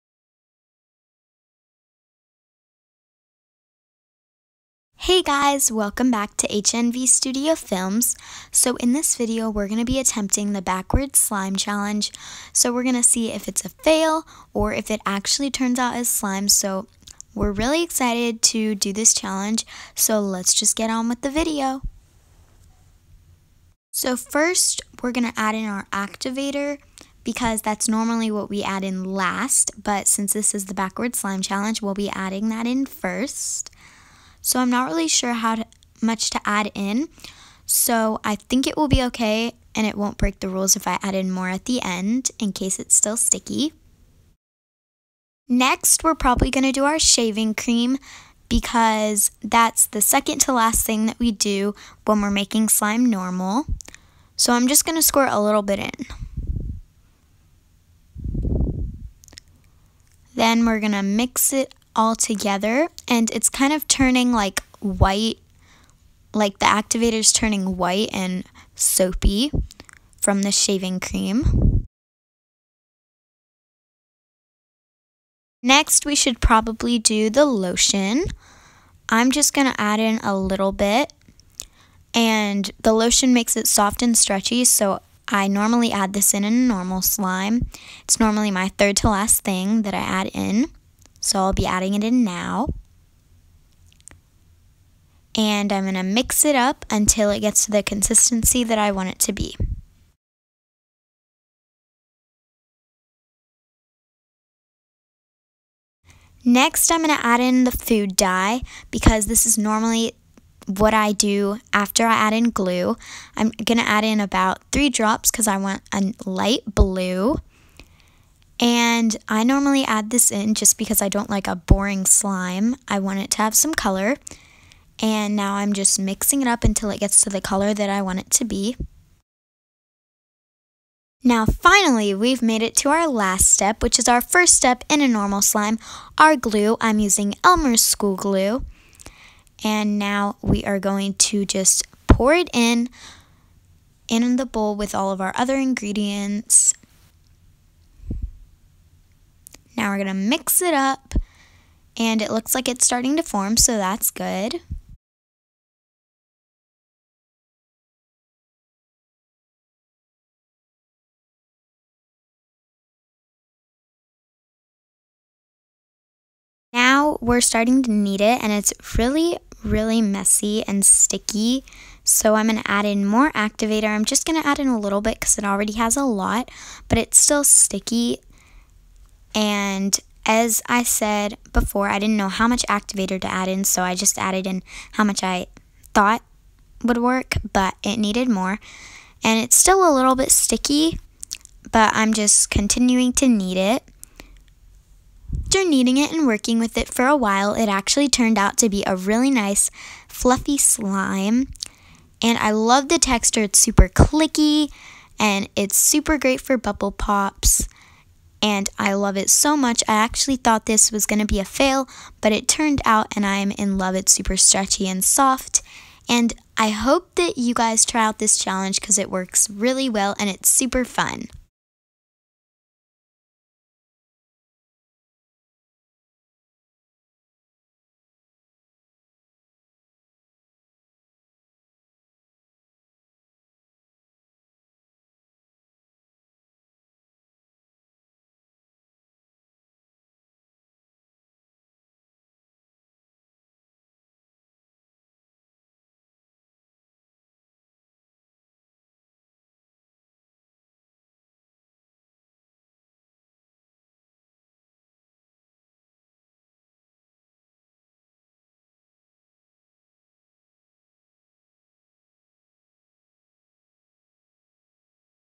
The Backwards Slime Challenge!
Hey guys, we are back with another video! In today’s video we do the backwards slime challenge! Do we Fail??